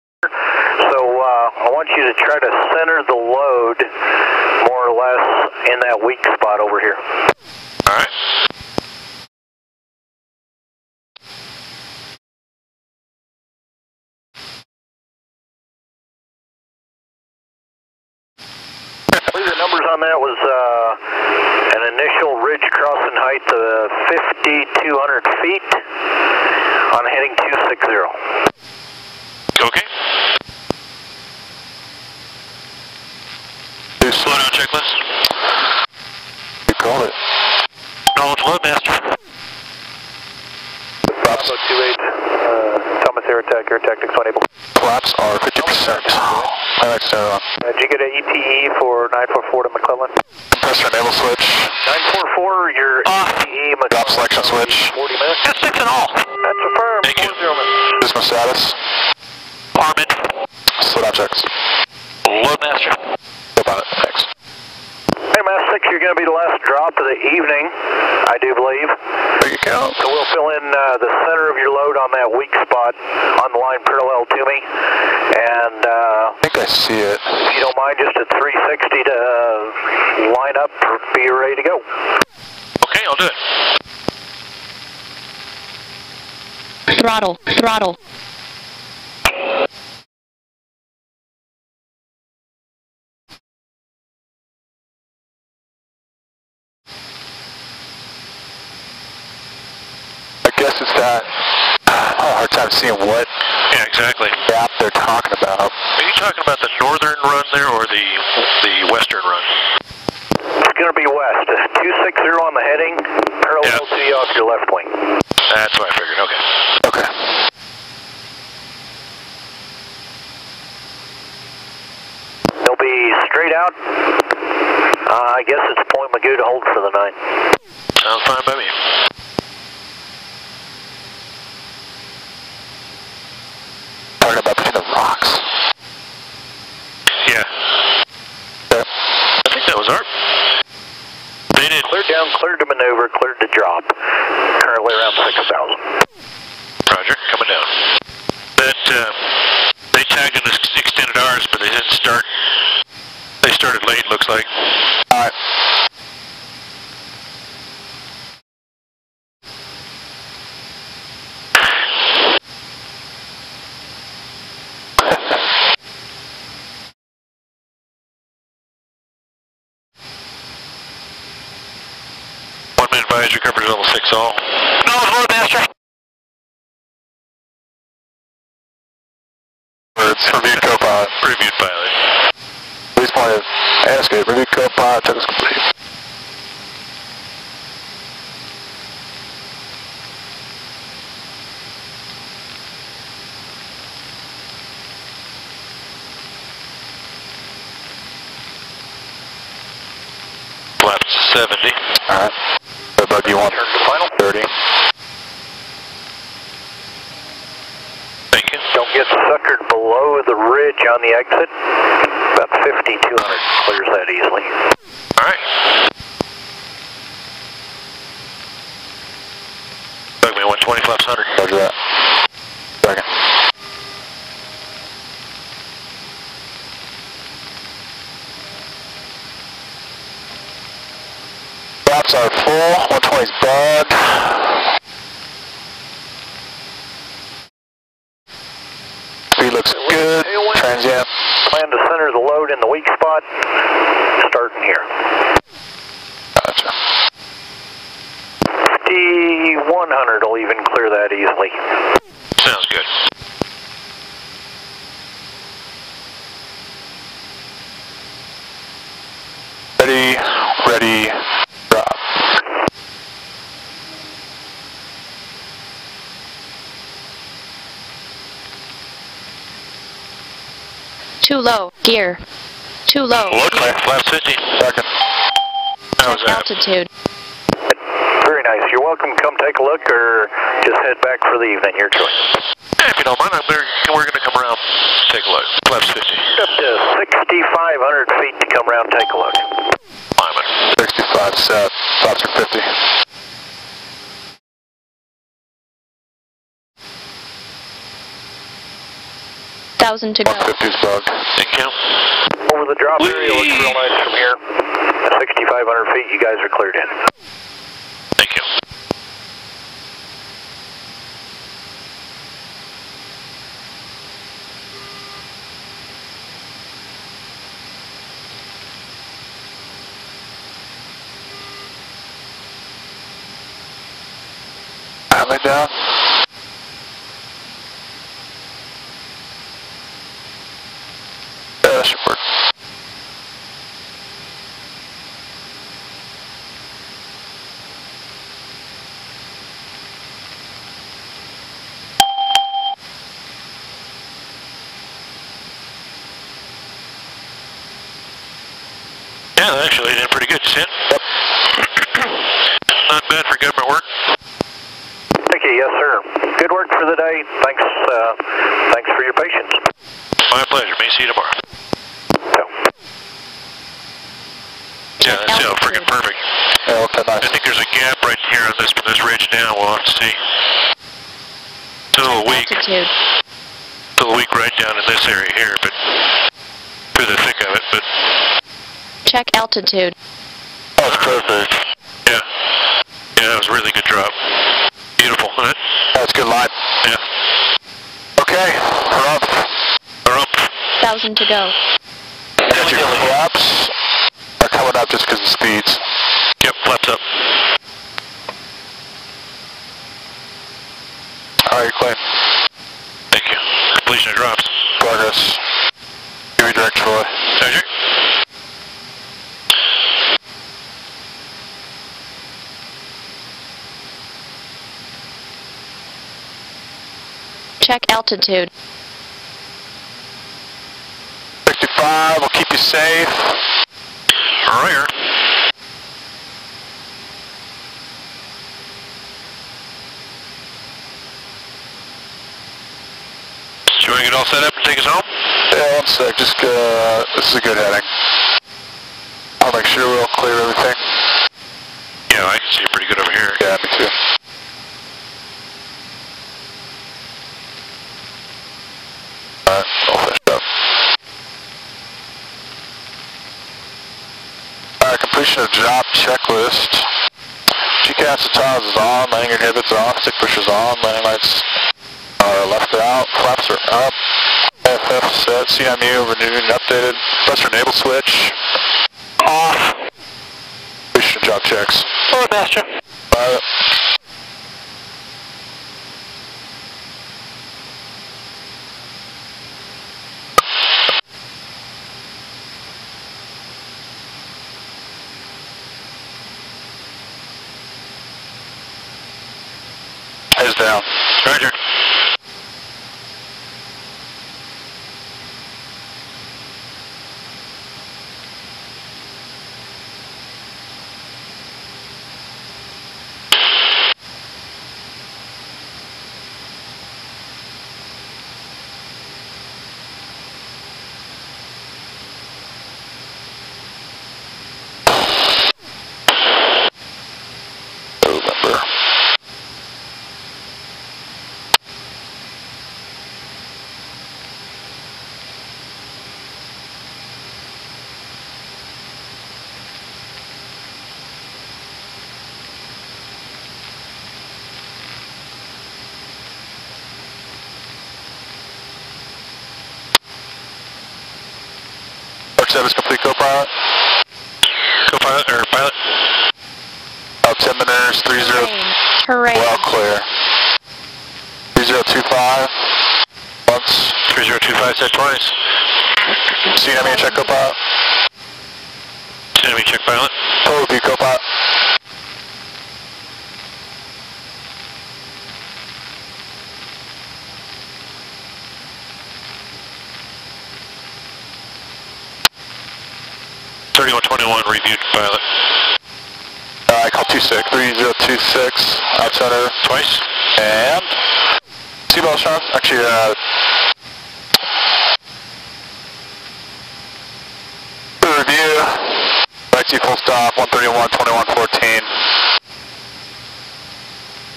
MAFFS6 Thomas Fire Ventura 8Dec17
MAFFS Aerial Firefighting on the Thomas Fire Dec 8, 2017. Filmed by Col Bryan Allen. The location of this drop area is Northwest of the City of Fillmore, Northeast of Ventura, on the larger mountain above Fillmore/Santa Paula